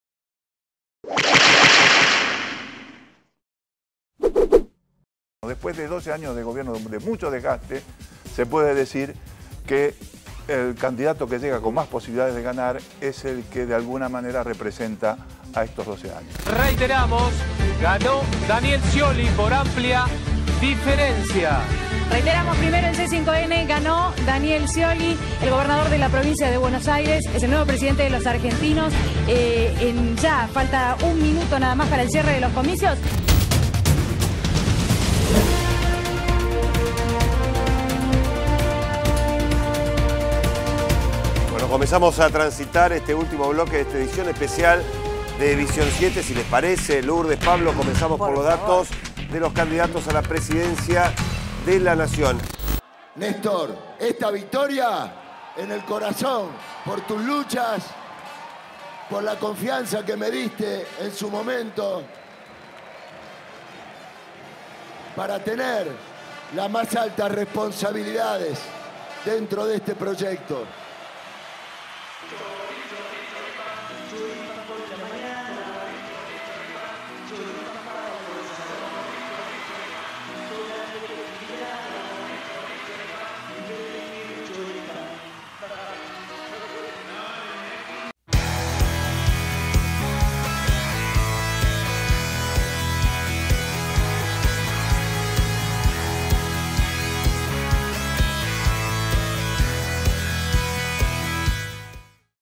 Scioli es electo Presidente de Argentina
Daniel Scioli es electo Presidente de los Argentinos por una abultada diferencia de votos.